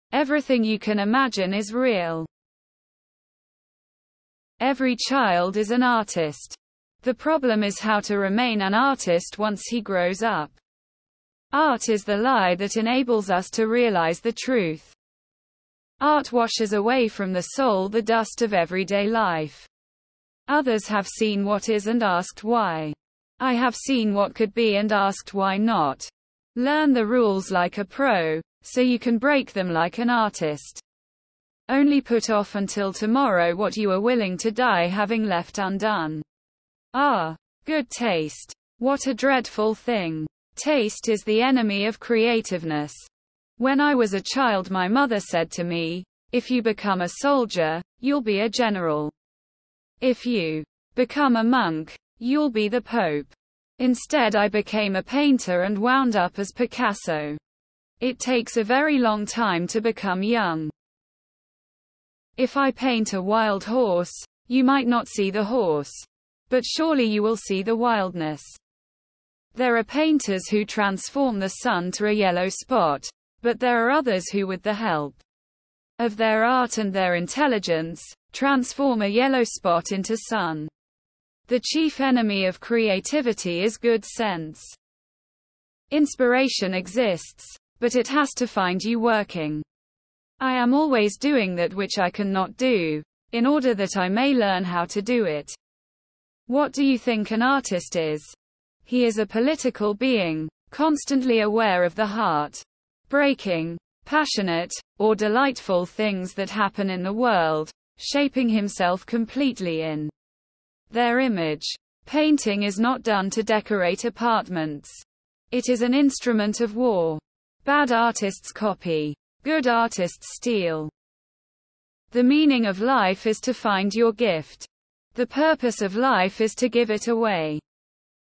Most colour full motivation quotes about life by Pablo Picasso | Voice of Wisdom
Pablo Ruiz Picasso(25 October 1881 – 8 April 1973) was a Spanish painter, sculptor, printmaker, ceramicist and theatre designer who spent most of his adult life in France. One of the most influential artists of the 20th century, he is known for co-founding the Cubist movement, the invention of constructed sculpture, the co-invention of collage, and for the wide variety of styles that he helped develop and explore. Among his most famous works are the proto-Cubist Les Demoiselles d'Avignon (1907), and the anti-war painting Guernica (1937), a dramatic portrayal of the bombing of Guernica by German and Italian air forces during the Spanish Civil War.